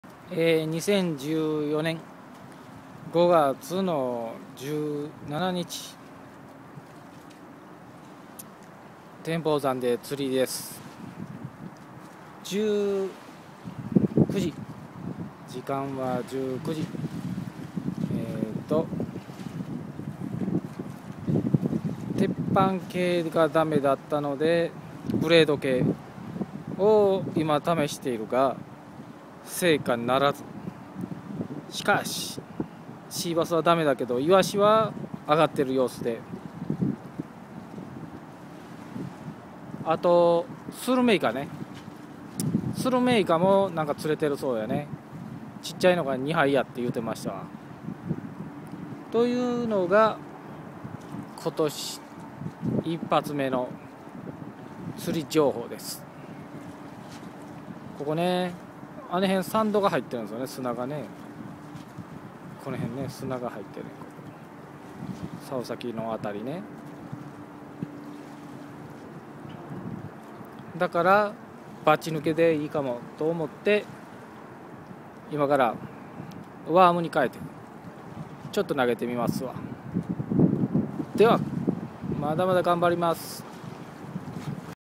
釣り情報 天保山 2014年05月17日
アジやイワシがあがってます。　バチヌケも良い感じです。　回遊系はまだ早い様子でした。  May 17, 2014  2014年05月17日